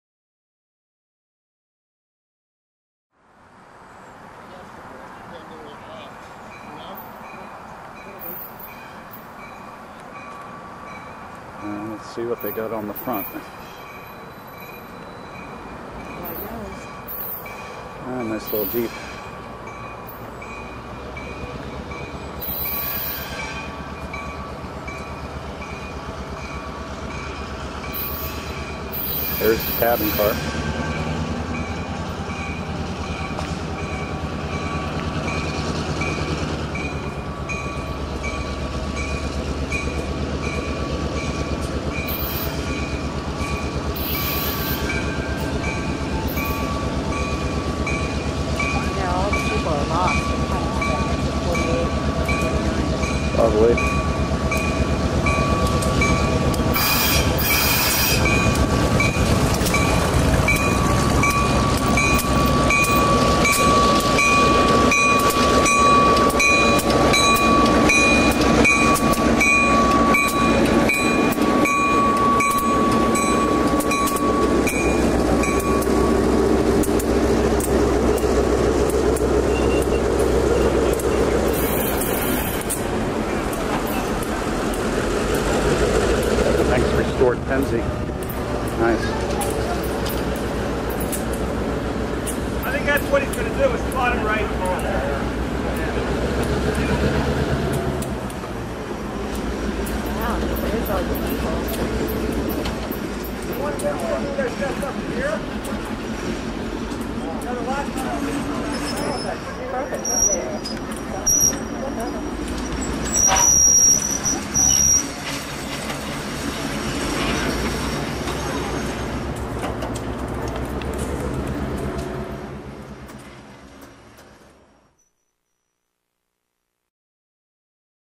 Railfest 2004: Our Transportation Arrives...
On the afternoon of October 2, we rode a short excursion on the Nittany & Bald Eagle Railroad out of Tyrone, PA.  A little sun had managed to part the clouds briefly, but they appeared to be regrouping as we waited for the excursion train to arrive.  Finally, well-dressed GP8 1602 appeared with its train.  This was actually a push-pull operation, with North Shore GP38 2004 out of sight on the other end.

Missed non-rail photo opportunity - the Tyrone station platform had a pay phone in a really funky late-60s/early-70s egg-shaped blue enclosure on a pole, with a heavily rusted but still visible Bell Telephone logo.